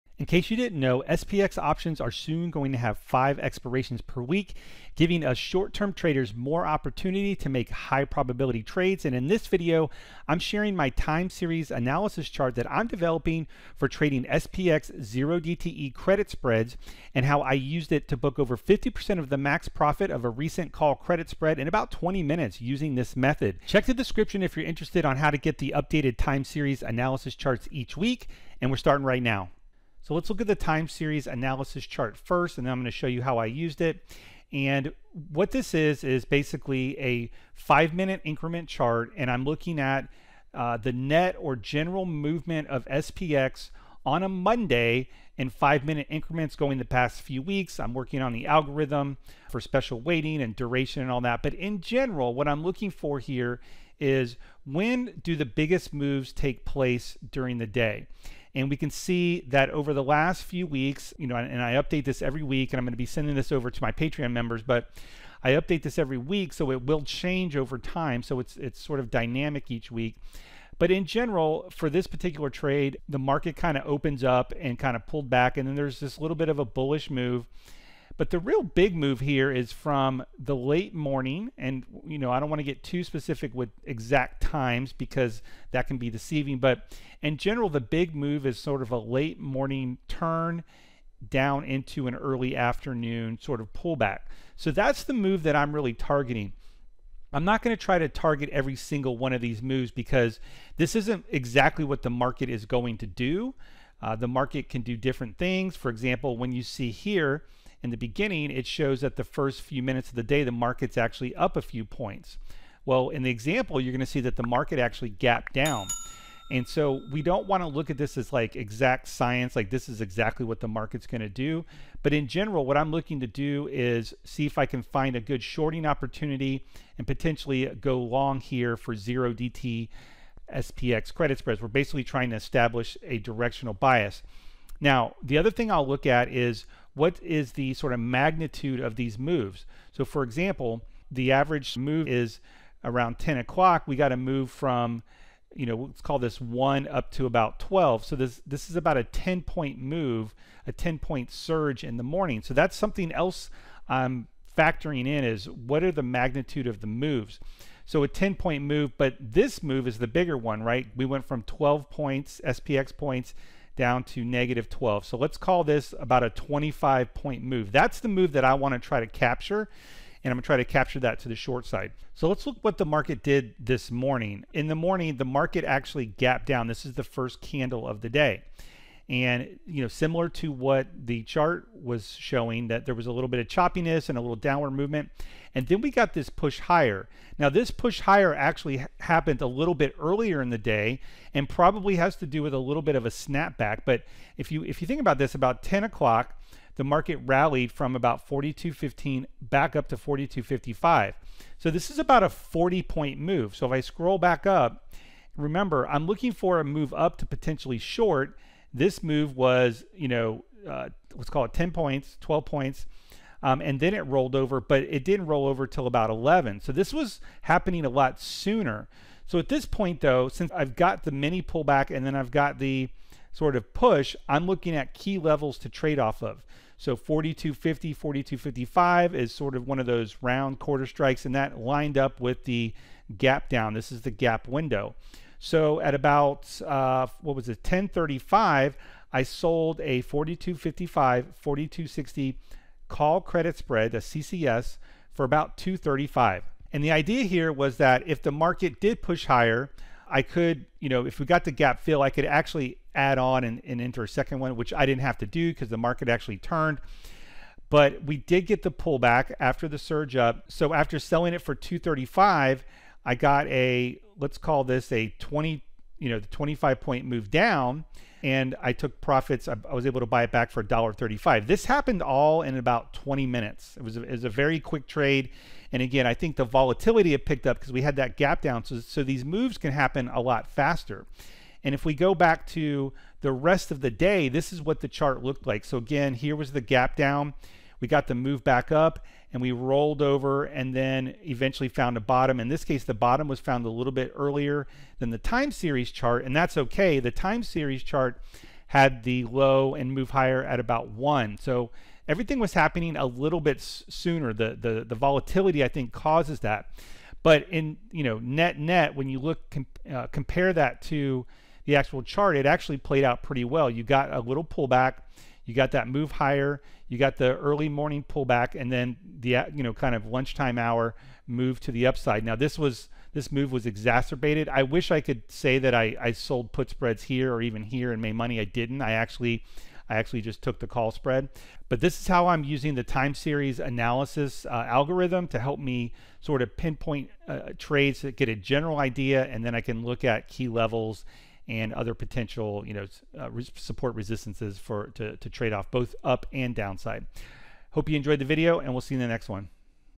SPX 0DTE Credit Spread Using Time Series Analysis
will receive the updated Time Series Analysis charts before the trading week starts to help SPX 0DTE traders establish a directional bias for their trades.

As shown in the video, the time series analysis chart helps find when the biggest moves may occur on a given day based on recent SPX price data from the CBOE. Key levels such as SPX quarter strikes are often good levels to trade off of as well as one's own technical analysis.

Here's another video using Time Series Analysis on the 30m time frame to help with timing 1-2DTE SPX credit spreads.

Option Omega is the best options backtesting platform for SPX and SPY.